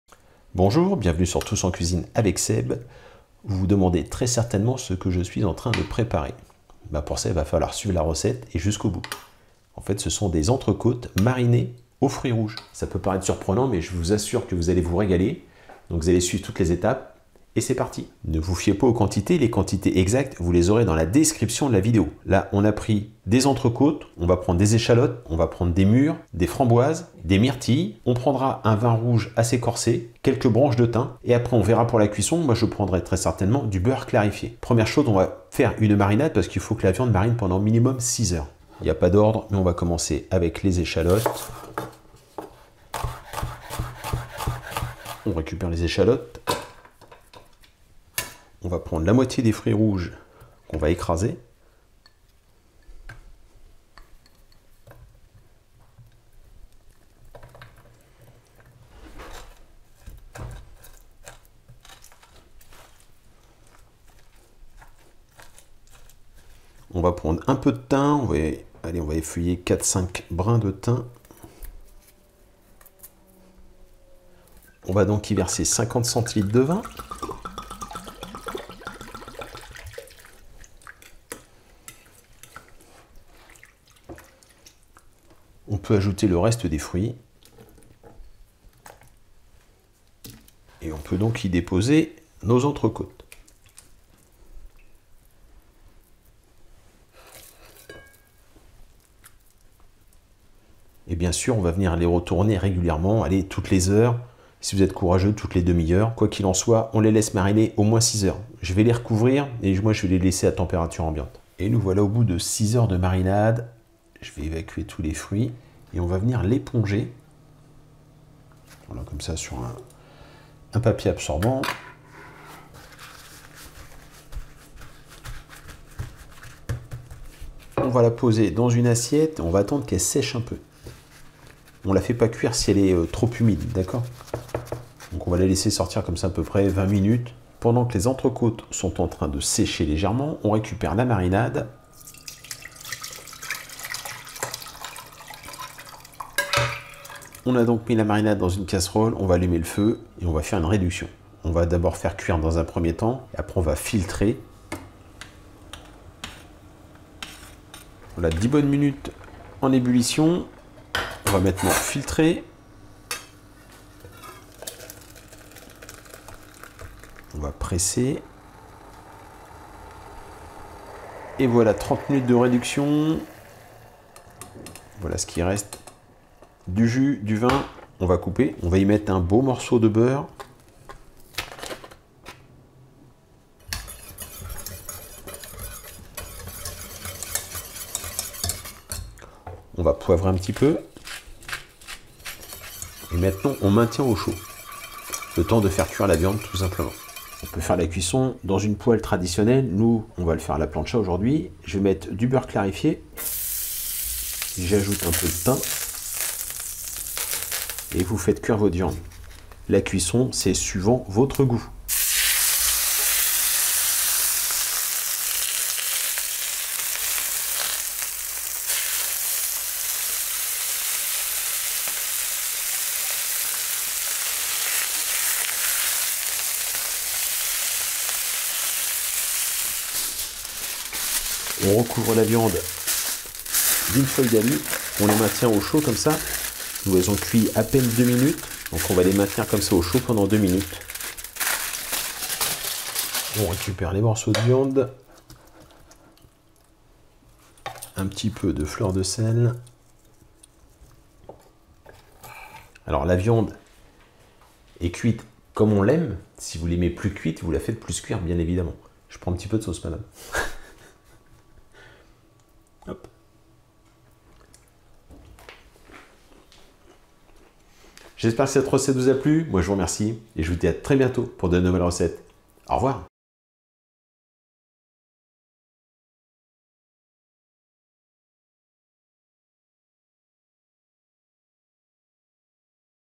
ENTRECÔTE MARINADE VIN et FRUITS ROUGE (Tous en cuisine avec Seb)
Entrecôte Marinée au Vin Rouge et aux Fruits Rouges : Une Symphonie de Saveurs pour un Barbecue Inoubliable !
Avis aux amateurs de viande rouge et de saveurs originales !
Préparez-vous à vivre une expérience gustative unique avec cette recette d'entrecôte marinée au vin rouge et aux fruits rouges.
Dans cette vidéo, je vous dévoilerai tous les secrets pour réaliser un plat digne des plus grands chefs.

Une viande tendre et savoureuse : une entrecôte de qualité, marinée dans une délicieuse marinade au vin rouge et aux fruits rouges.
Des saveurs intenses et raffinées : le vin rouge apporte une note corsée et légèrement sucrée, tandis que les fruits rouges ajoutent une touche de fraîcheur et d'acidité.
Cette recette est idéale pour un barbecue entre amis ou en famille.
Elle saura surprendre vos invités et leur faire vivre un moment gourmand inoubliable.
Suivez les étapes de ma vidéo et laissez-vous guider pour réaliser une entrecôte marinée au vin rouge et aux fruits rouges digne des plus grands restaurants.
Bon appétit et à vos barbecues !

1 pièce de bœuf de 800 g sans os (ou une côte de bœuf de 1,2 kg)
4 échalotes pelées
5 brins de thym + quelques-uns pour la cuisson
125 g de framboises
125 g de mûres
125 g de myrtilles
50 cl de vin rouge corsé
40 gr de beurre